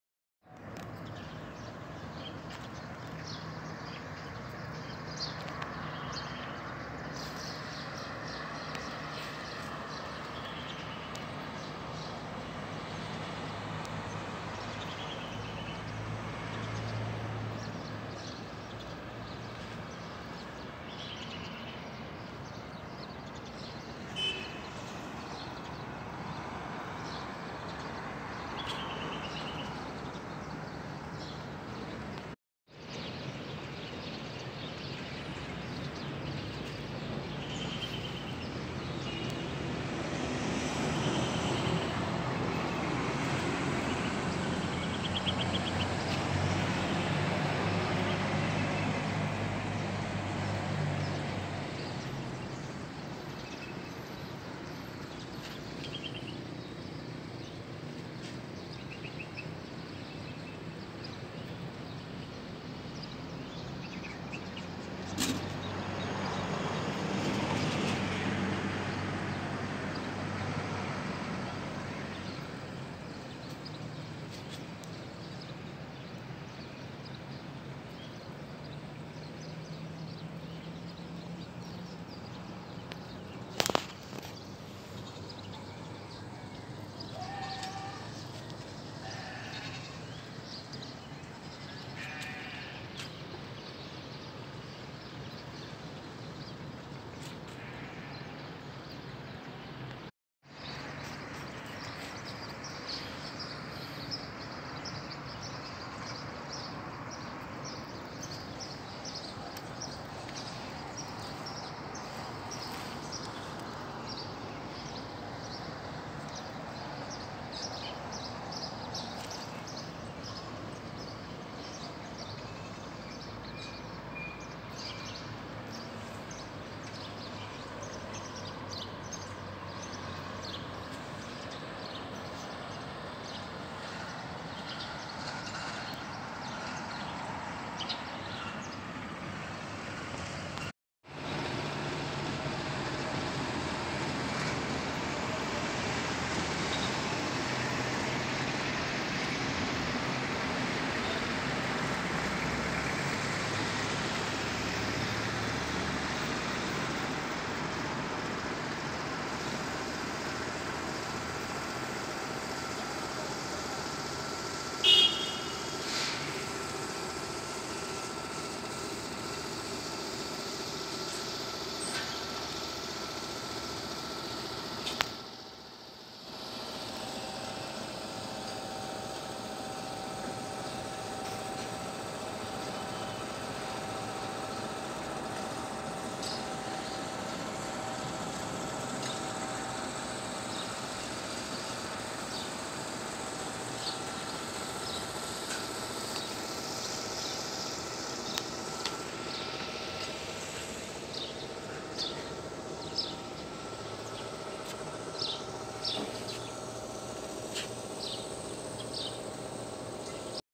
beauty of sieyun
very nice old city beautiful sieyun in Yemen raidha to sieyun 400 KM way is very high 🗻 mountain 🏔️